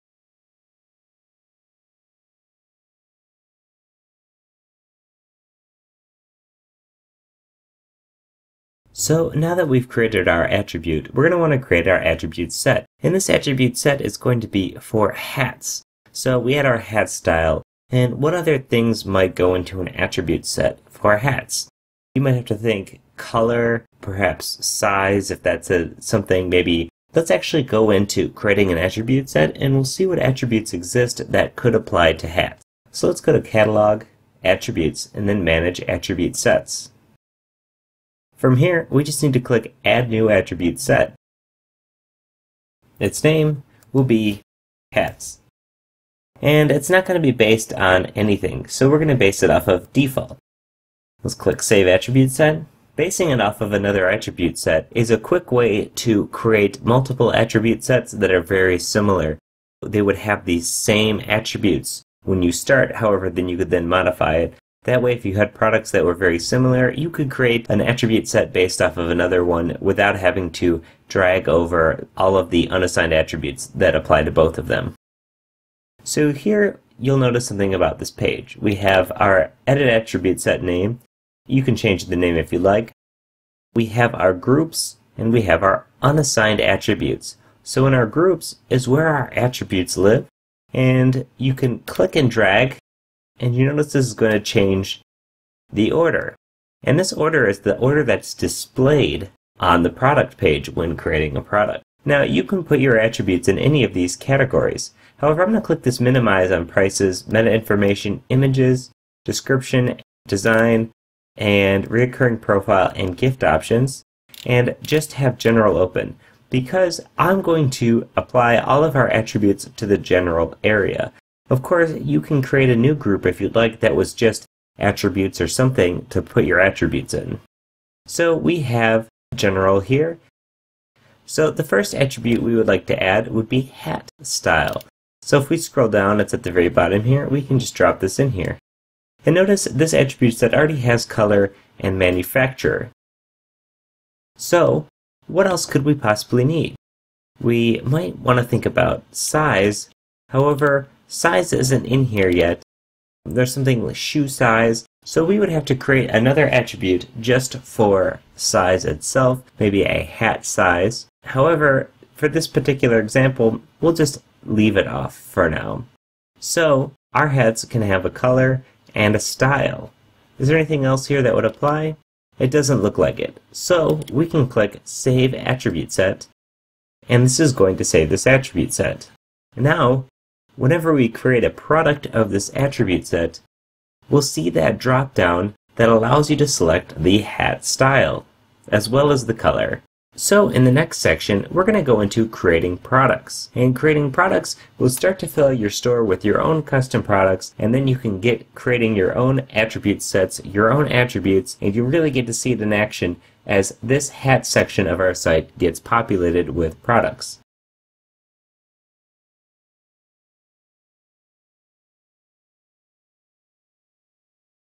Getting Started with Magento Tutorial: Creating Attribute Sets | packtpub.com
Magento only comes with a limited number of premade attributes sets. Here we create our own and add attributes to the set.
• Go to the attribute set page
• Create a new attribute set
• Add the attributes and save them